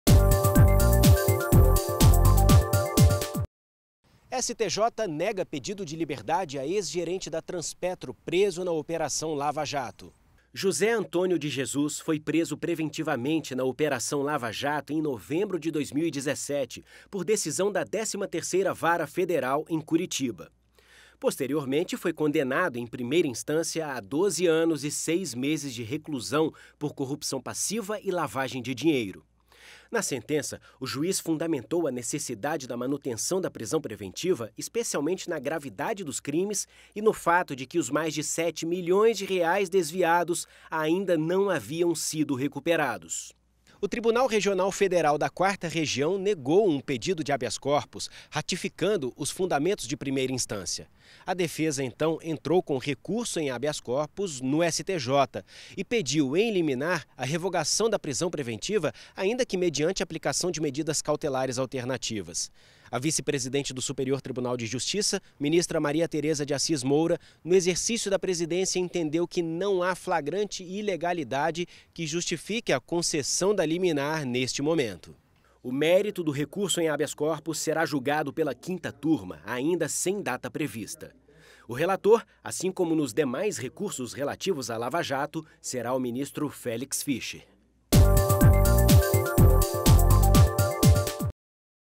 Clique STJ - Transpetro (12/07/2019)
STJ nega pedido de liberdade a ex-gerente da Transpetro preso na Operação Lava Jato